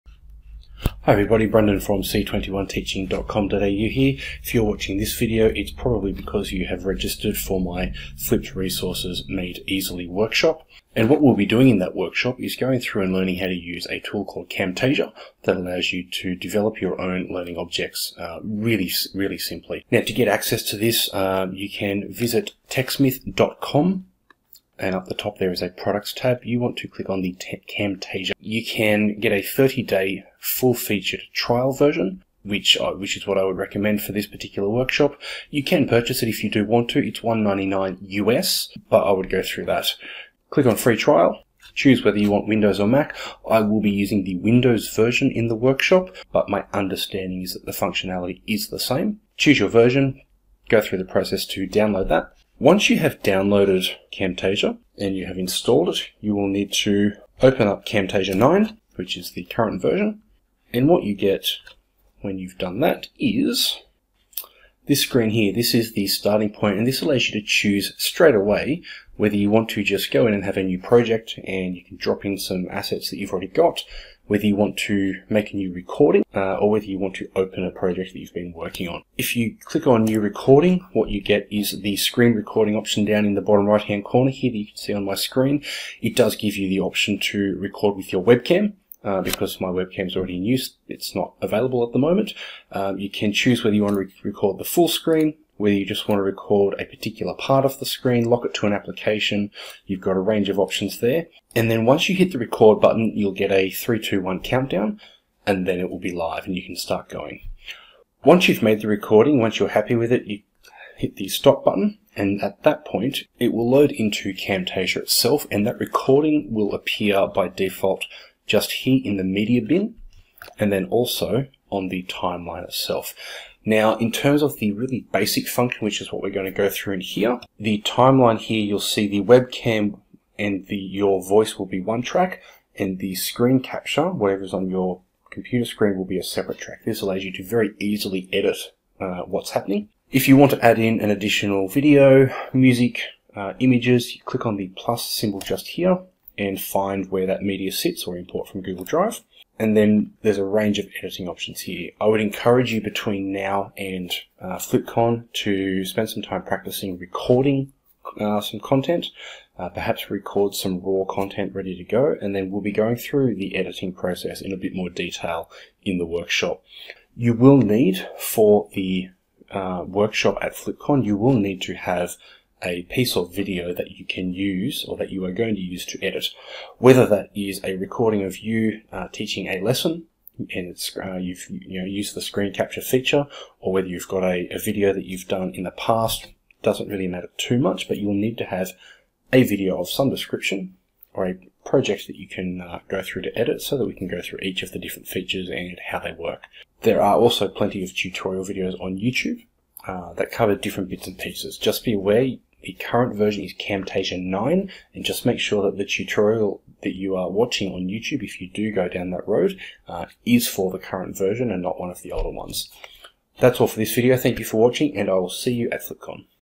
FlipConAus2017 Pre Learning: Flipped Resources Made Simply
This video is part of the pre-learning in my Flipped Resources Made Simply workshop at FlipCon Australia in October 2017.

This video provides a quick guide to finding, downloading, installing and then some basic functions of Camtasia which is the resource we will be using in the workshop.